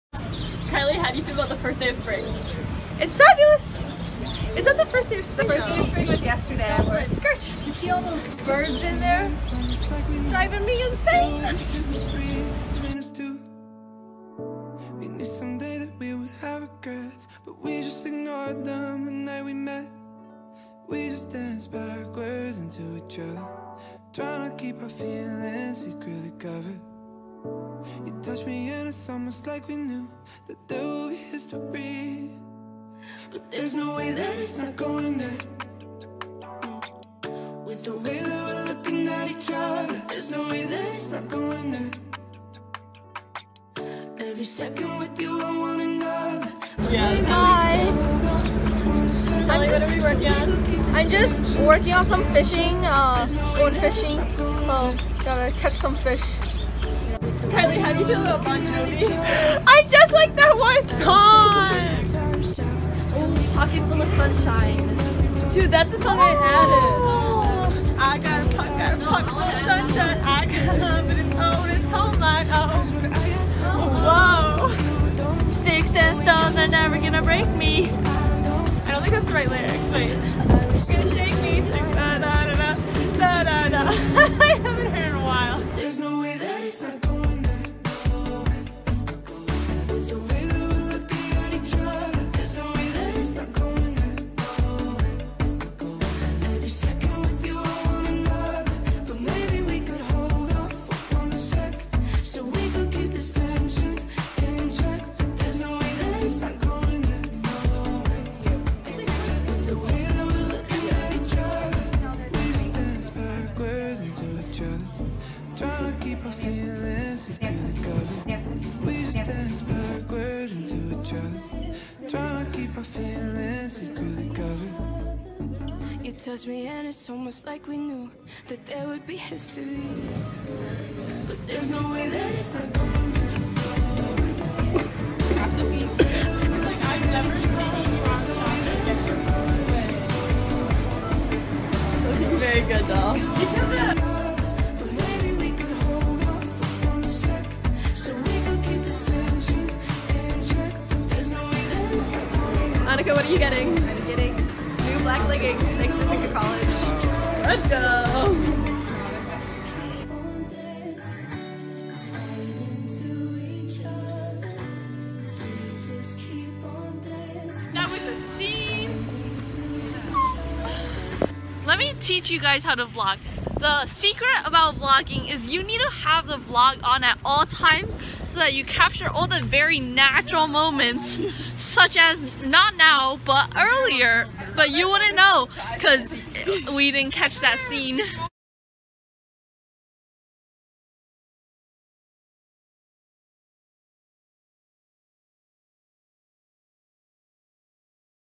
Vlog 2: Off Campus and On Newbury Street
watch as we spend a friday after classes hanging out on Newbury Street! It's just a 15 minute walk from campus and has shops, cafes, and plenty to do.